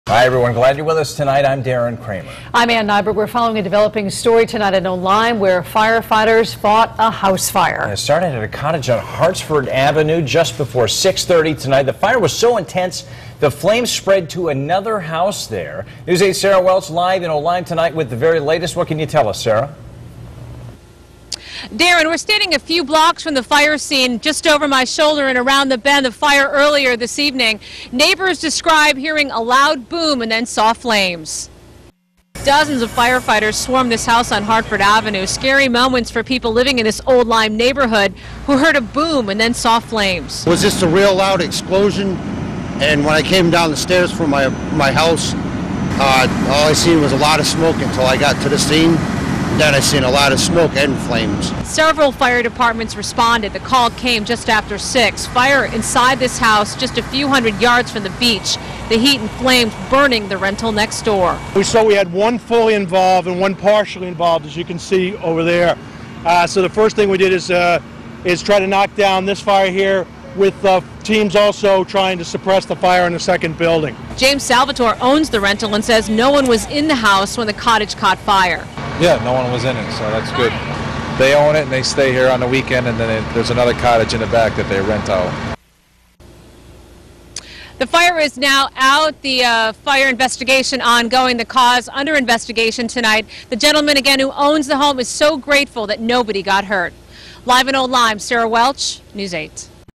Explosion burns two Old Lyme cottages
Old Lyme firefighters are investigating a fire that burned two seasonal cottages on Hartford Avenue Wednesday evening.